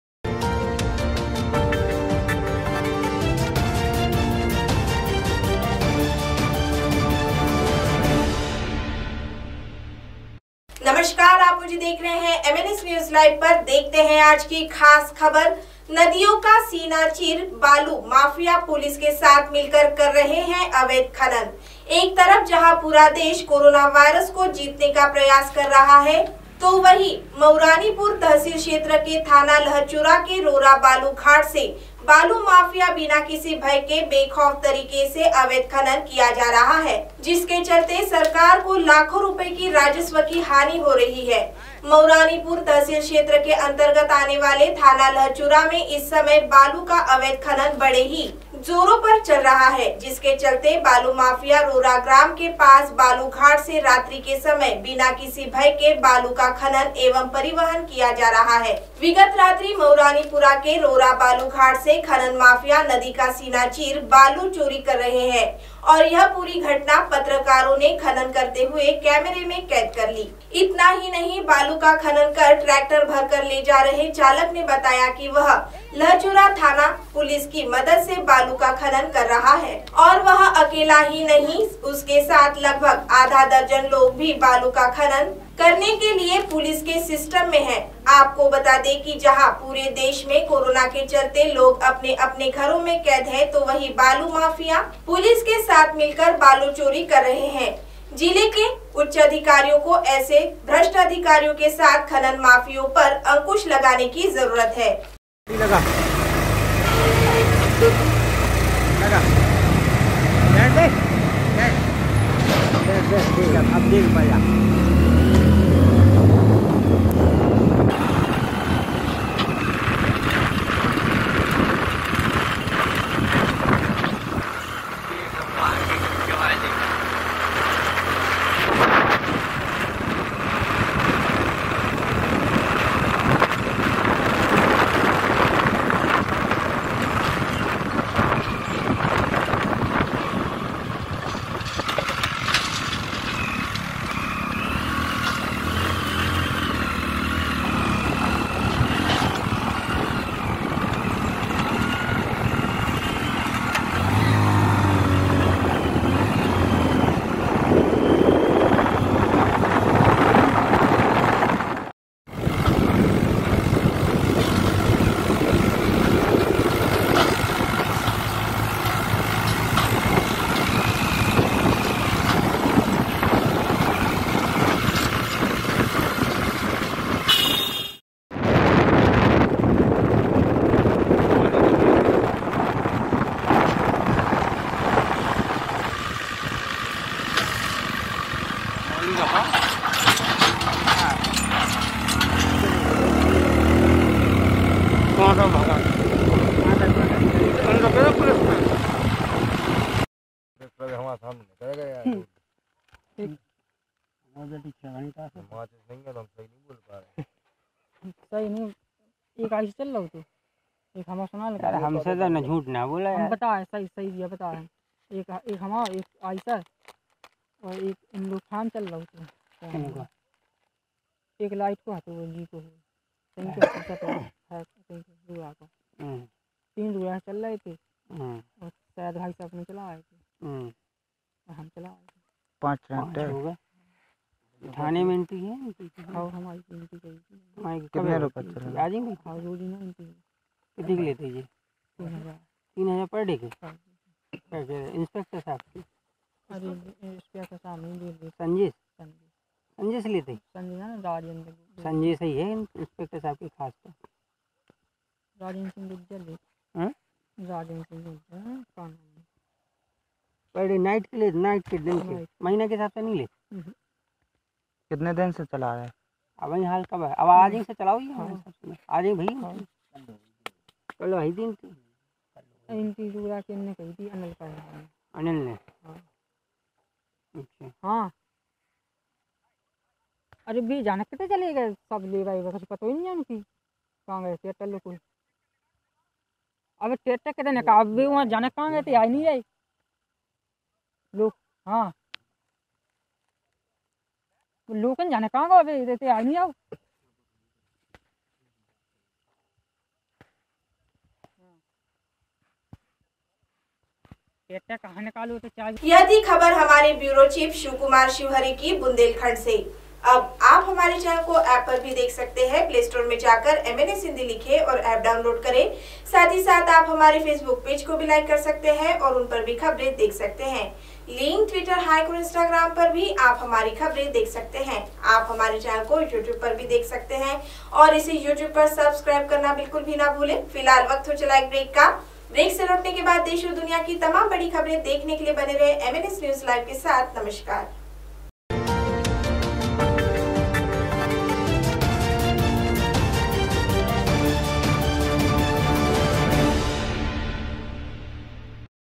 का सीना चीर रेत माफिया पुलिस के साथ मिलकर कर रहे अवैध खनन।
MNSNEWSLIVE नदियों का सीना चीर रेत माफिया पुलिस के साथ मिलकर कर रहे अवैध खनन।